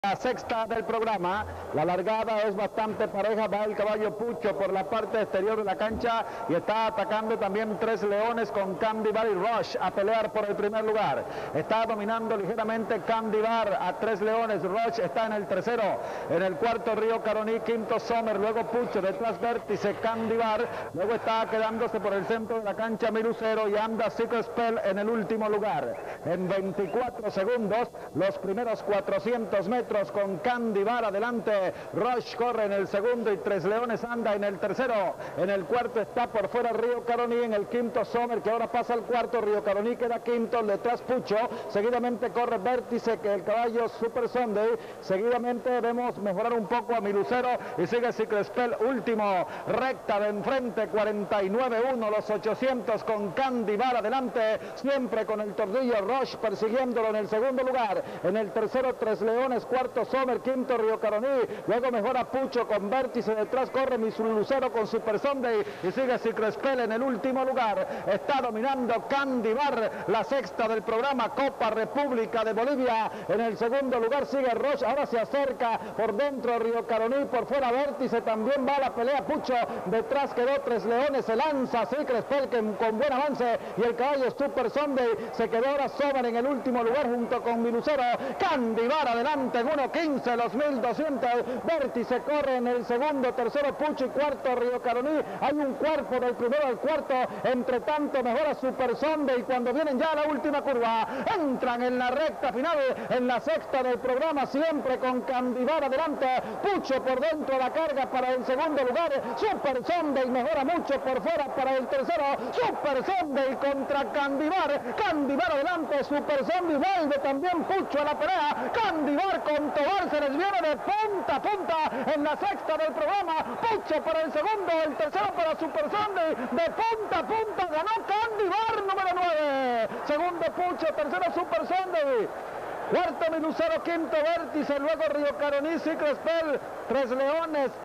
🔅 Carrera Comun 1986 CANDY BAR VH 9 🔅
Jinete: J.V. Tovar
Entrena: V. Lombardi
Narra: Hector A. Rivas